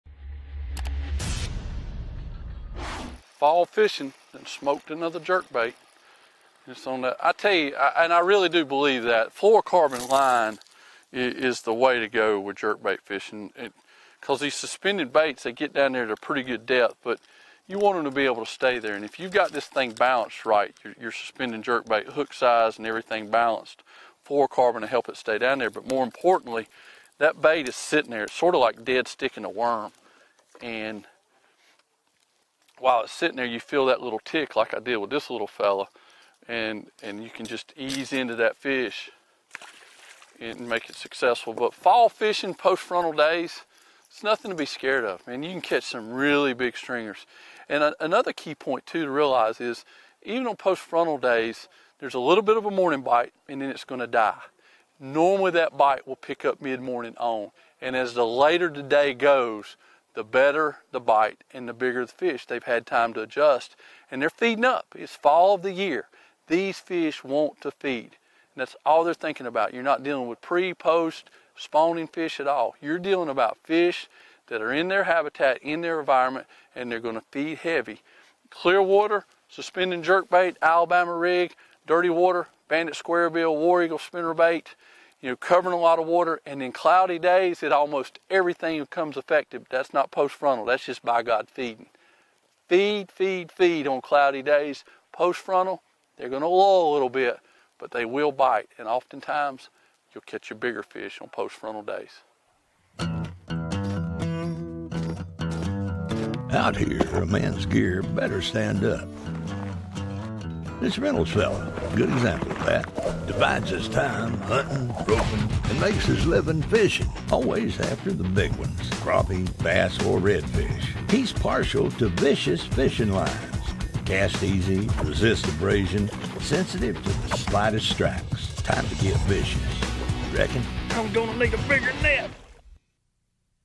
Fishing on Post Frontal Days
Marty Stone talks bass fishing with jerkbaits and why big bass can be caught on post frontal days.